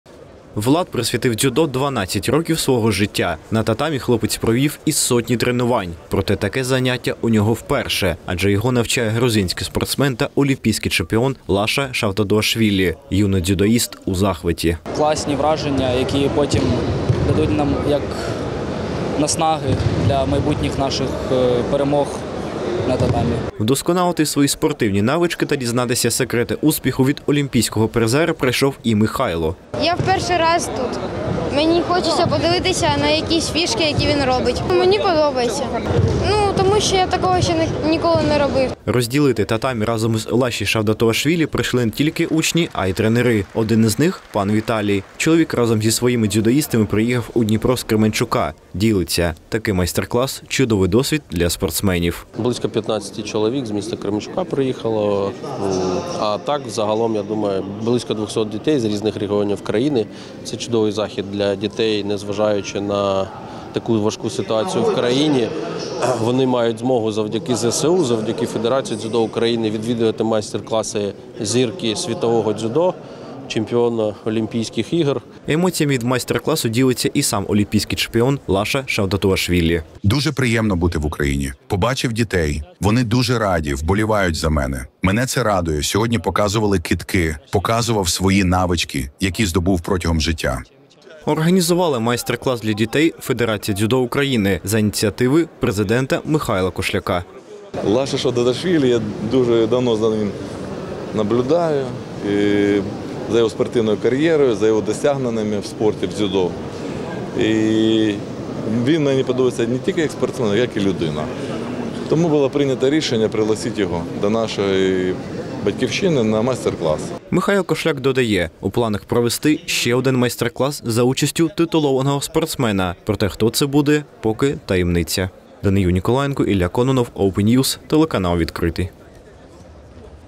У Дніпрі пройшов майстерклас олімпійського чемпіона з дзюдо Лаша Шавдатуашвілі. Що кажуть учасники?
🥋Дізнатися секрети успіху та вдосконалити свої навички. До Дніпра приїхав олімпійський чемпіон з дзюдо Лаша Шавдатуашвілі. Прибув він для того, щоб провести для наших спортсменів майстерклас. Подробиці - у відео.